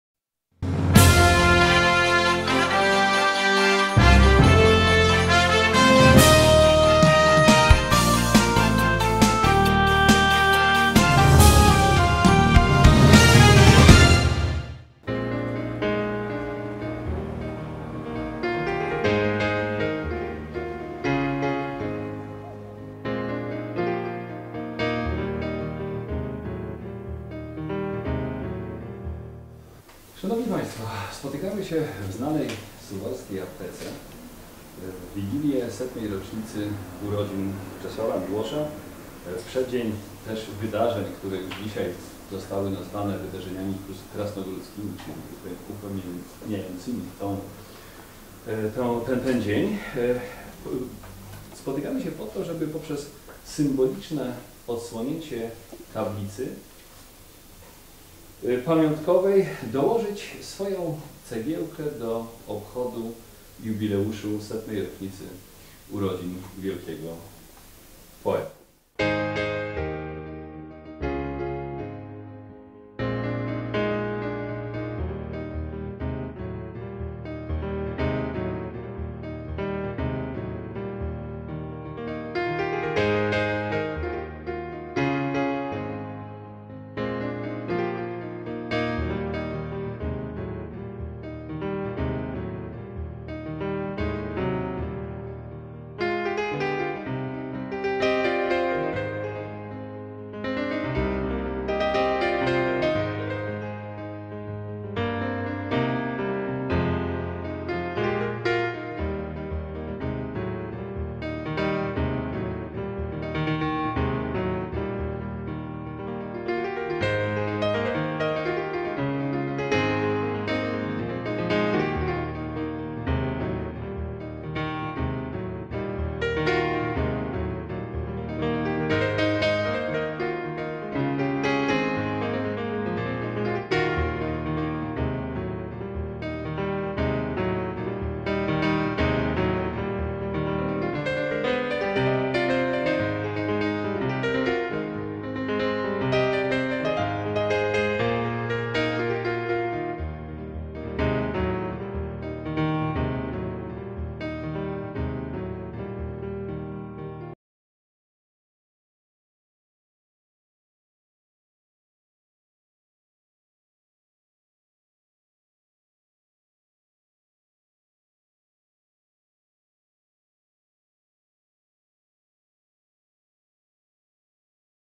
Odłonięcie tablicy poświęconej Czesławowi Miłoszowi
W aptece przy ul. Kościuszki odsłonieto tablice pamiatkową